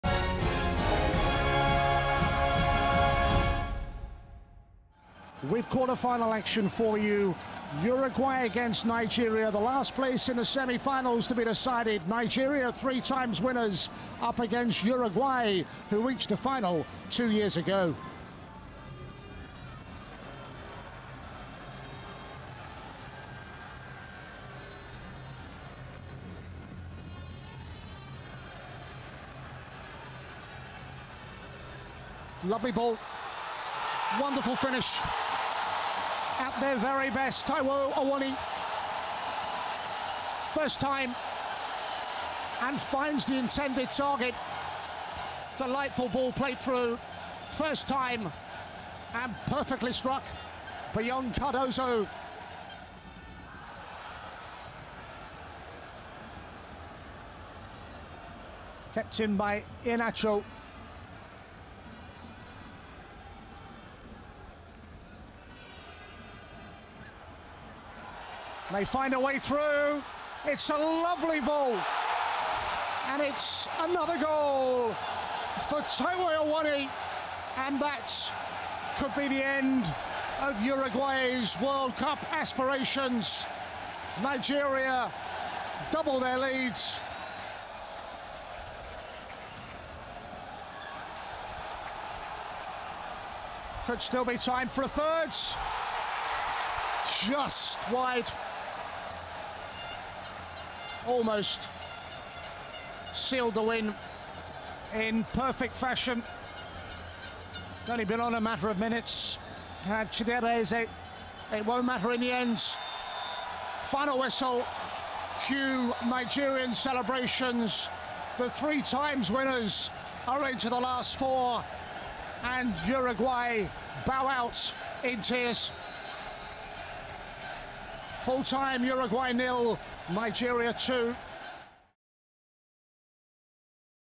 Awesome Awoniyi powers Nigeria
Uruguay - Nigeria, FIFA U-17 World Cup UAE 2013: Taiwo Awoniyi scored a brace as the impressive Nigerians brushed aside the powerful Uruguayans.

FIFA U-17 World Cup UAE 2013, Sharjah, Sharjah Stadium, Quarter-finals, 1 November 2013. Full match details: